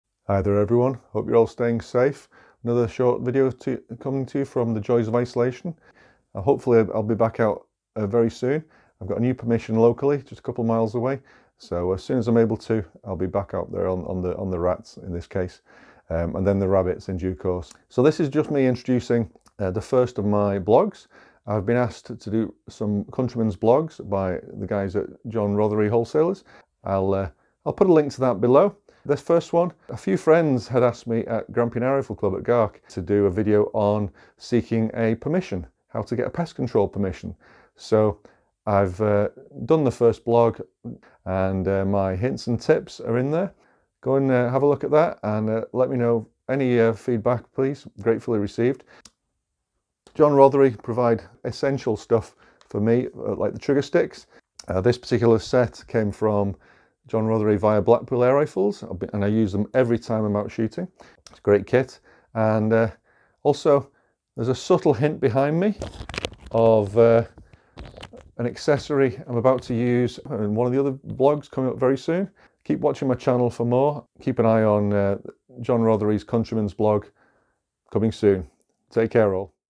001 Seeking a Permission
Good afternoon all, here I introduce the first of my blogs, for John Rothery Countryman's Blog, my advice on seeking your first pest control Permission
Take care and I'll be back very soon with more reviews, blogs and more pest control too.
Cheers. Russ